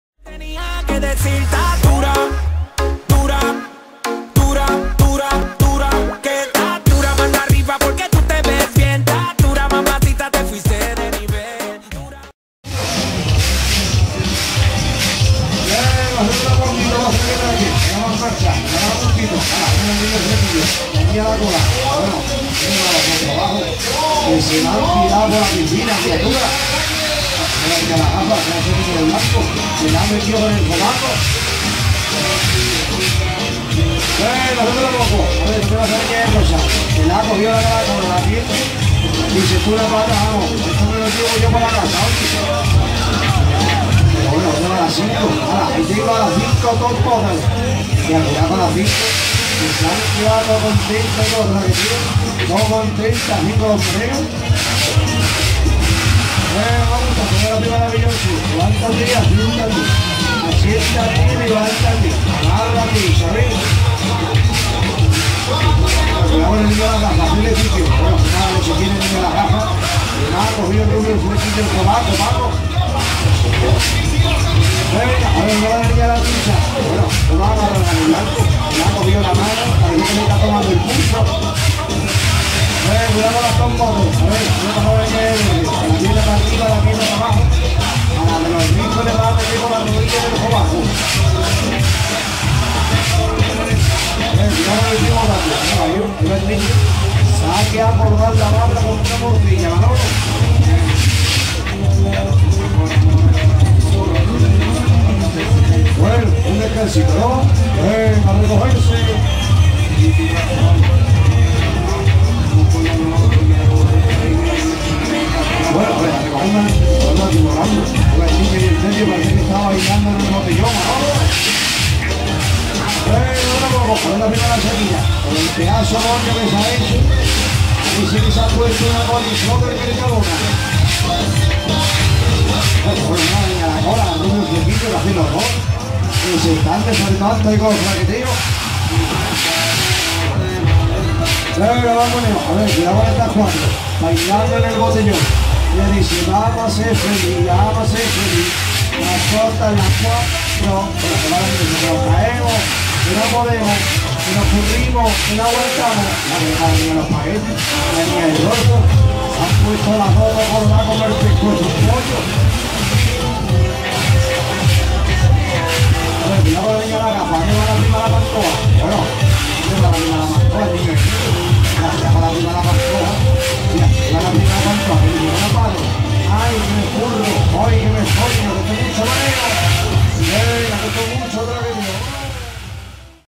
Atracción The King OFFRIDE Feria Cártama 2018
La atracción The King atracc. Cristo offride en la feria de Cártama (Málaga) 2018.